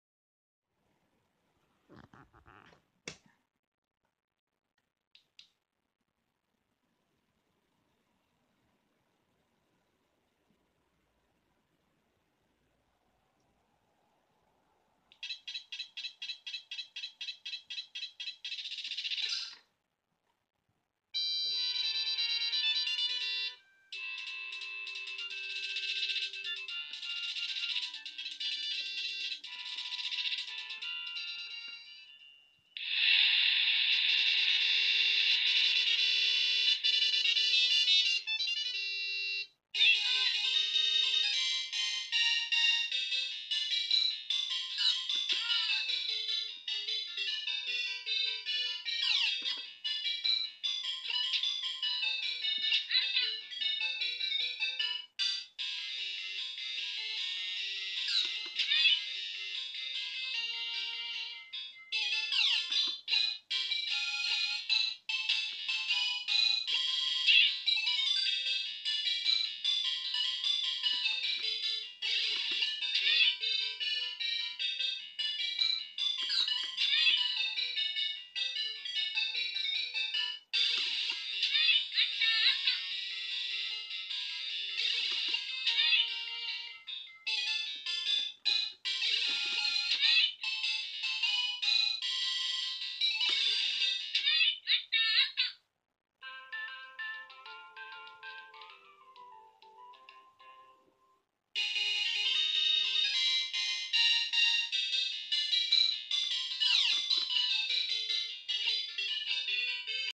System 1 (Namco) CPU board repair #4
Symptoms : sprites are missing

FIx : replaced a bad 2k  8-bit SRAM @H1 and three faulty 74LS173 @D3-E3-F3